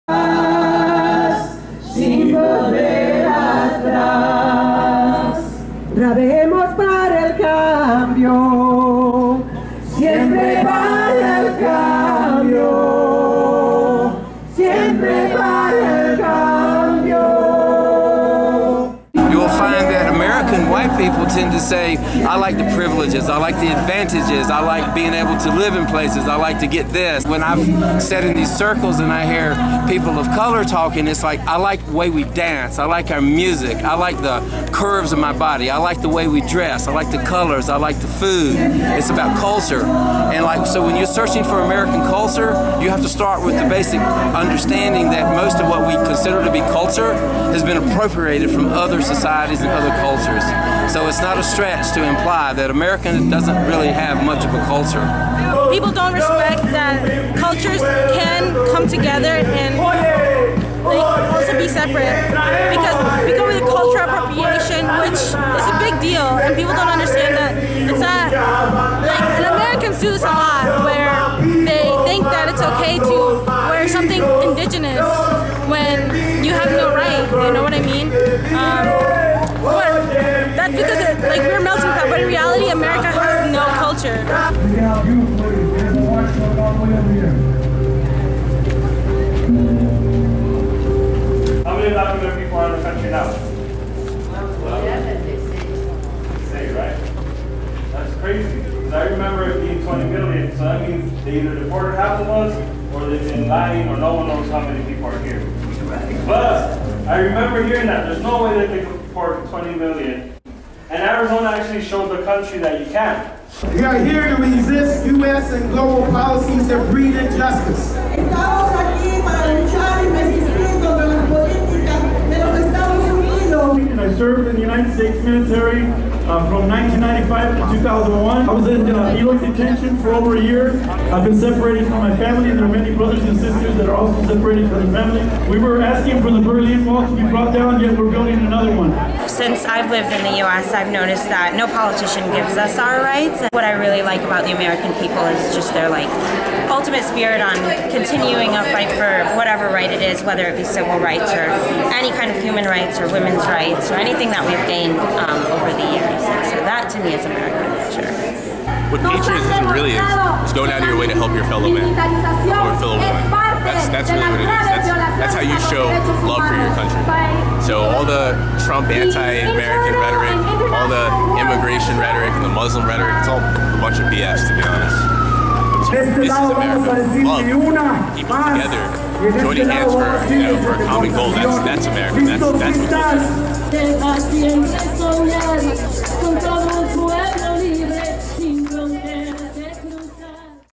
What is American culture at the Mexican border?
"What is American culture?" People standing next to our border wall with Mexico answer this question. For the Arizona week (October 2-9) of Cultures in Harmony's "What is American culture?" project, CiH founder William Harvey went to the US/Mexico Border Convergence, organized by SOAW